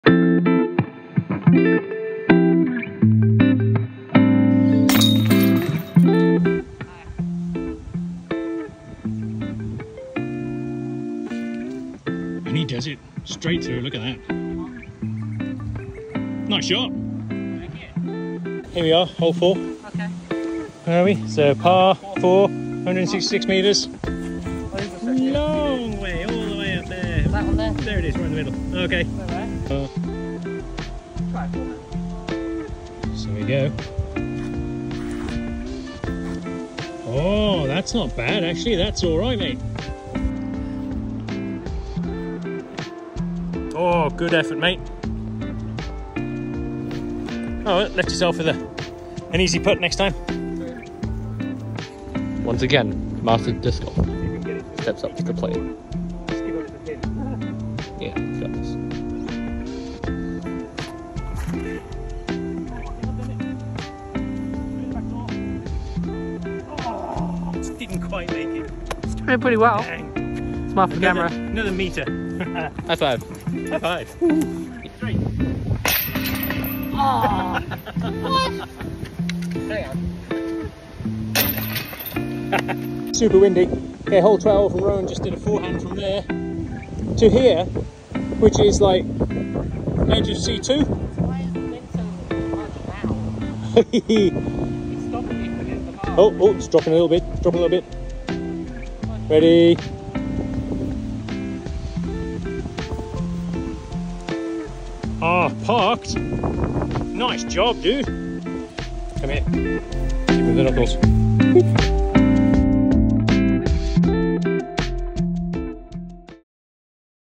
Disc Golf Day
An unexpected disc golf day with Rohan. Had a great time despite the wind swirling around Cadogan Park